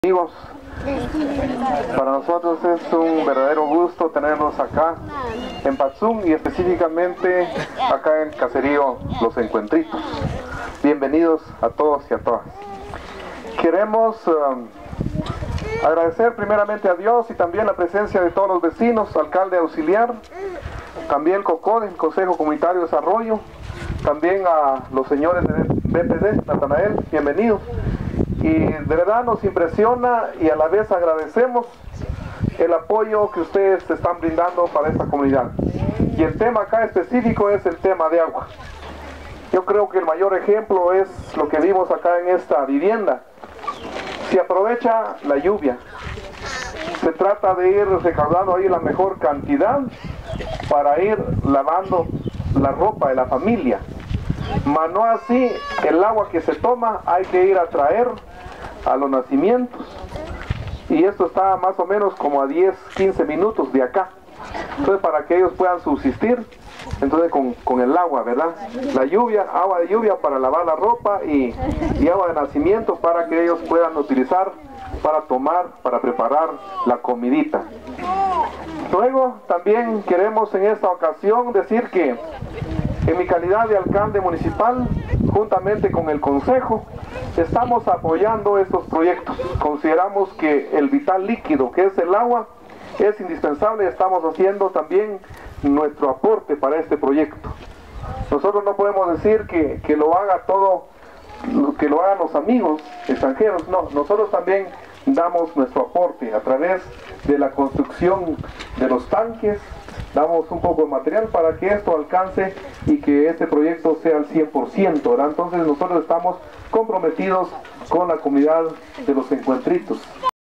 Palabras Alcalde en visita BPD
Patzún, Palabras Alcalde en visita BPD de voluntarios de Inglaterra para evaluar los proyectos apoyados en Los Encuentritos